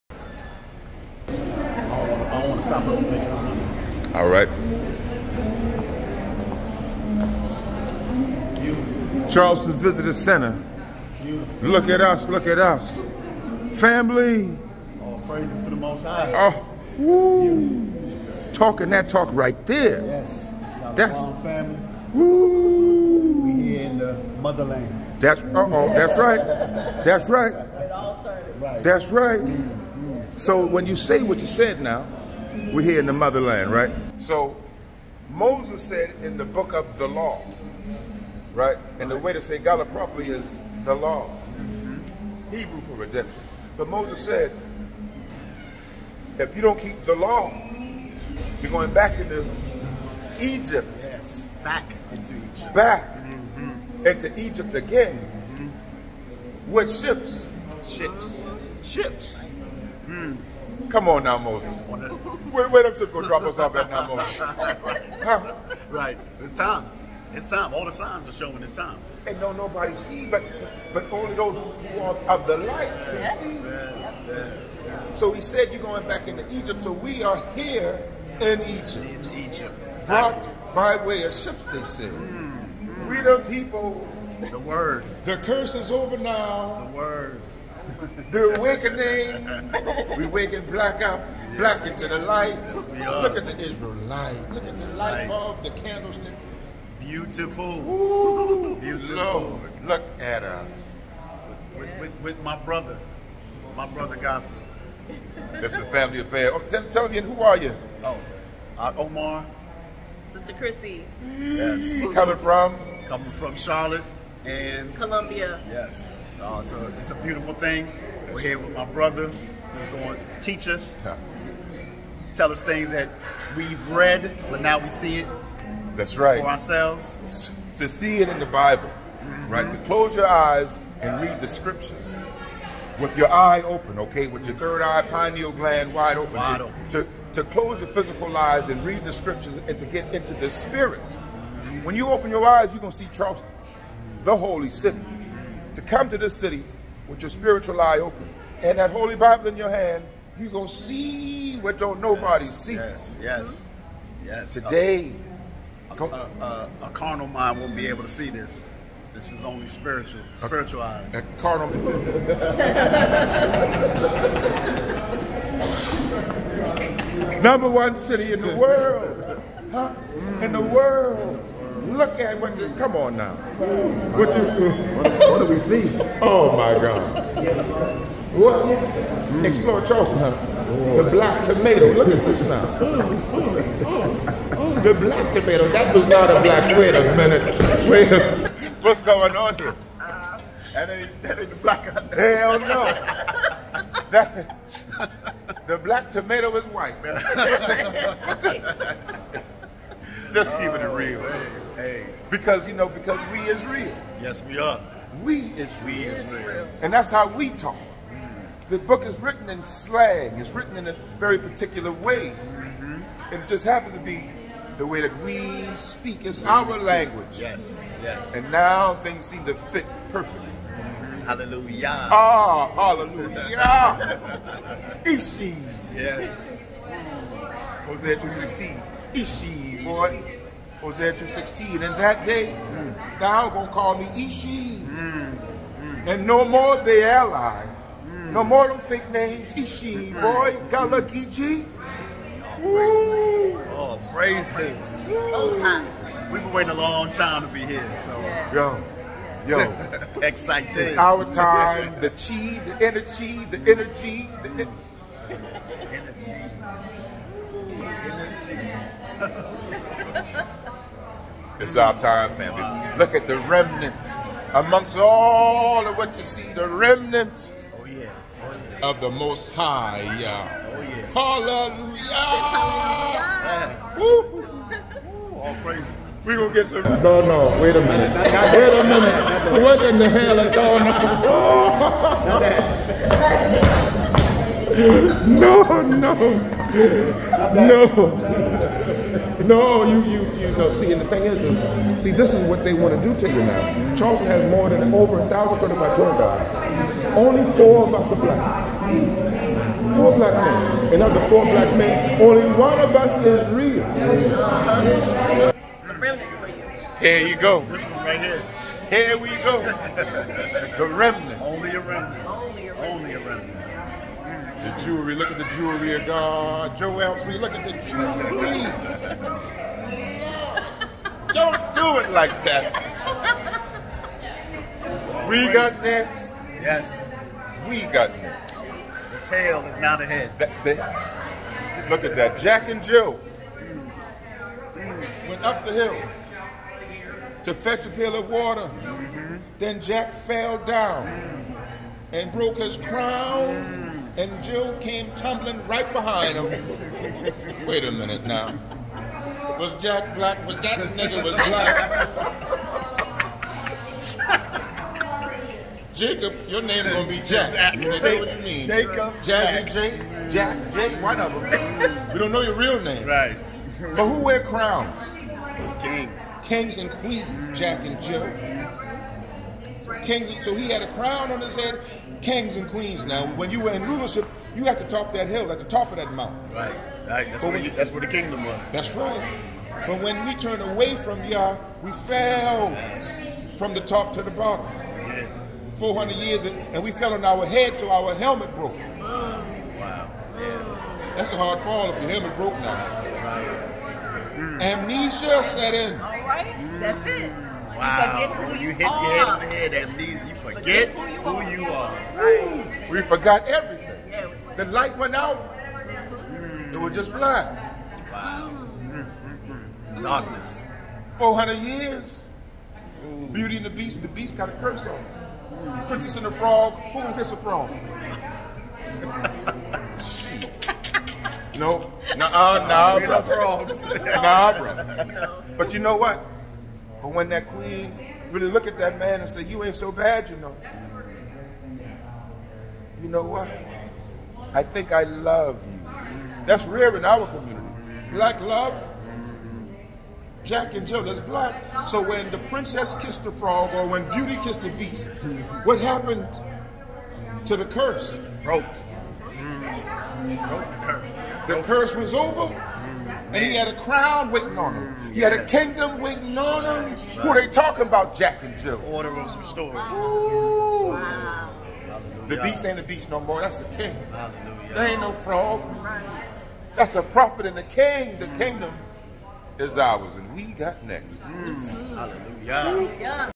GODFREY GULLAH JACK THE BLACK TOUR GUIDE HIDDEN JERUSALEM EGYPT WITH SLAVE SHIPS HEBREW ISRAELITE
Unveil Charleston's Soul: A Gullah Geechee Historical City Walk with the Gullah Geechie Chief Godfrey KHill.

Step back in time and walk in the footsteps of resilience on this unique Gullah Geechee historical city tour. Immerse yourself in the rich tapestry of Charleston, woven with threads of struggle, faith, and cultural brilliance.